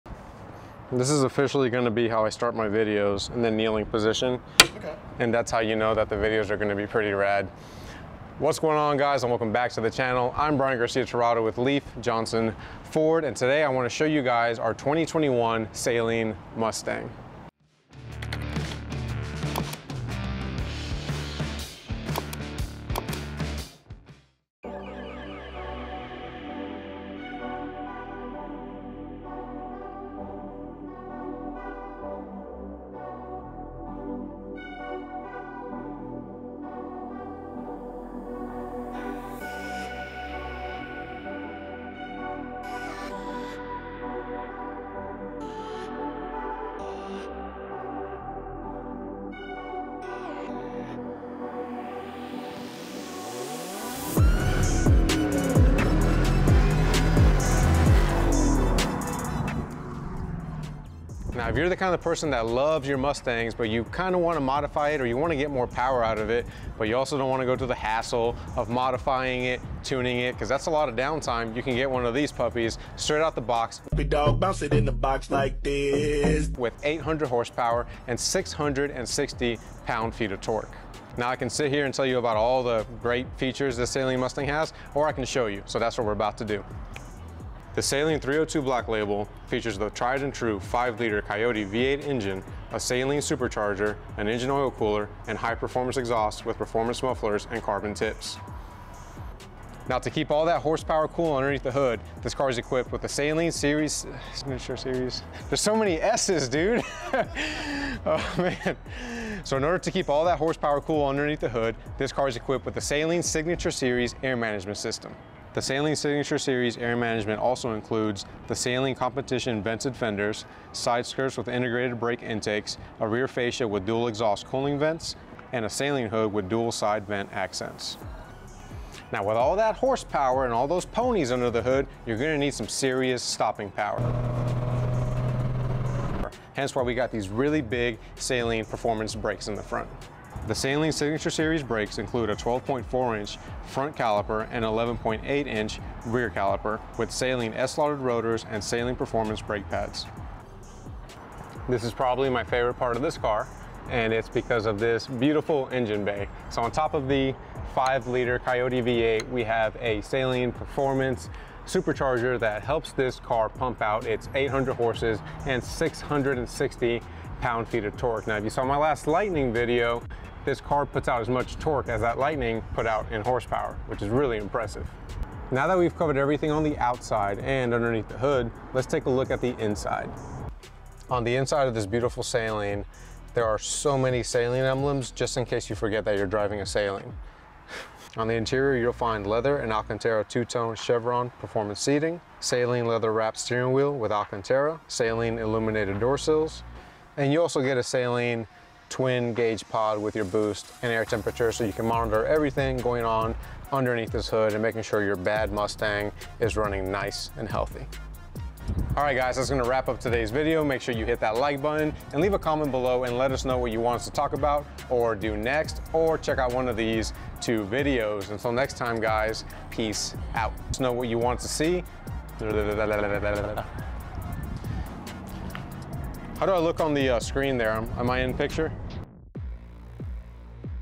A Closer Look At A 2021 Saleen Black Label Mustang ( IT RIPS!)
In today's video I go over our 2021 Saleen Black Label Mustang!

This crazy pony is equipped with a Saleen supercharger that allows it to pump out 800 horsepower and 660 pound feet of torque!

There is a lot that goes into giving the Mustang the Saleen name and this car has it all. Carbon fiber aerodynamic parts, bigger wheels and brakes, oil coolers and larger radiator and the list goes on!